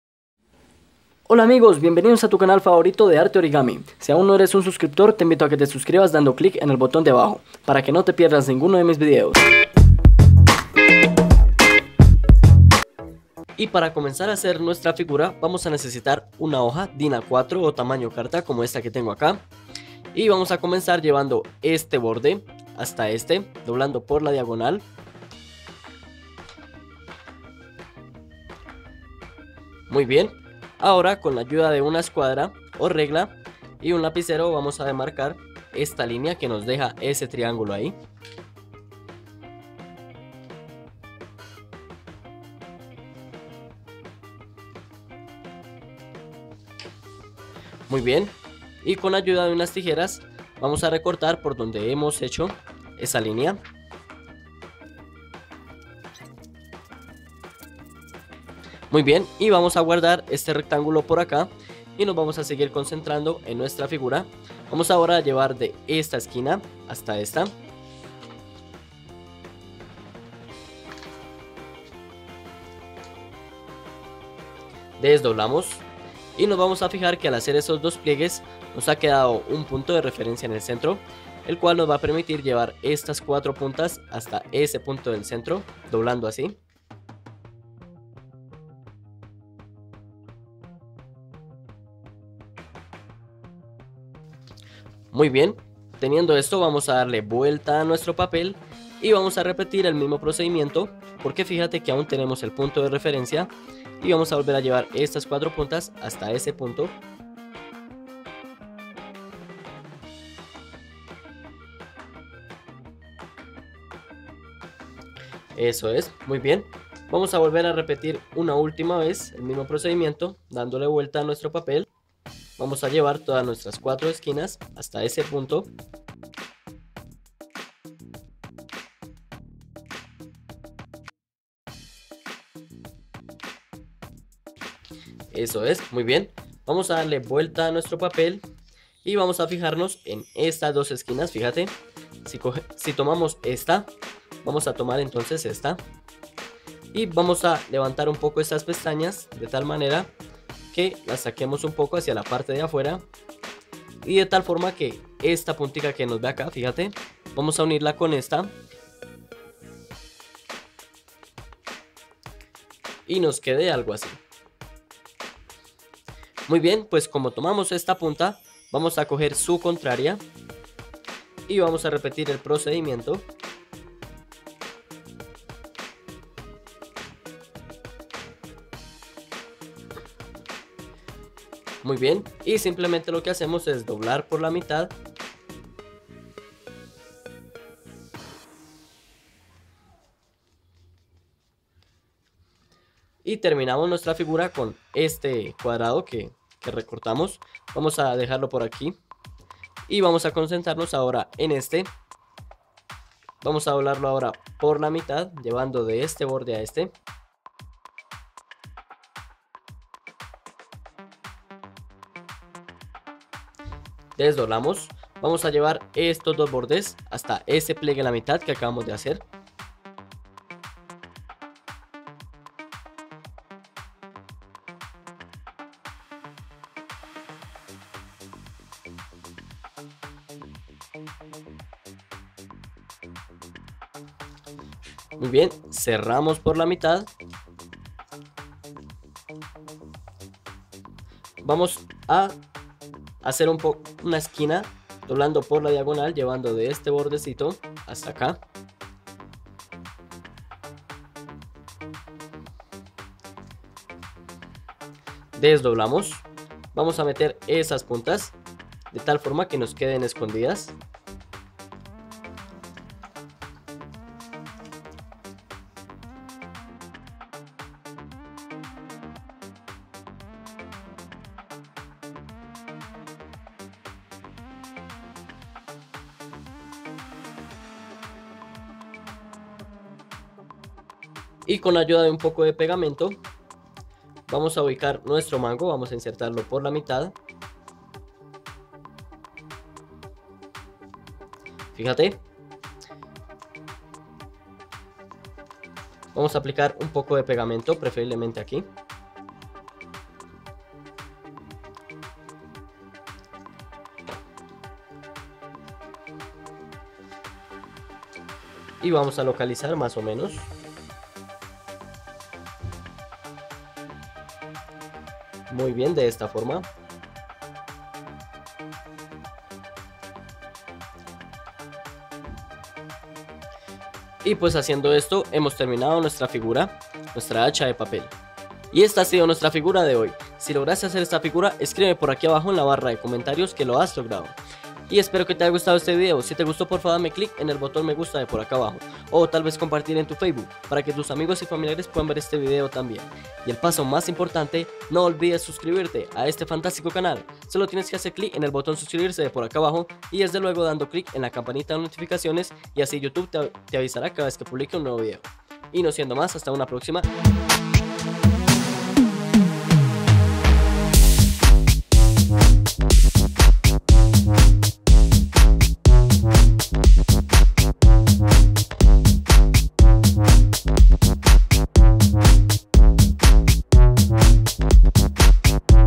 Como hacer un HACHA de papel - how to make a paper ax
Hola amigos, en este tutorial he querido enseñarte a hacer un hacha de papel ¡¡muy facil de hacer.!, los materiales son 1 HOJA de papel tamaño carta, y lo mejor de todo!
ATT: Arte Origami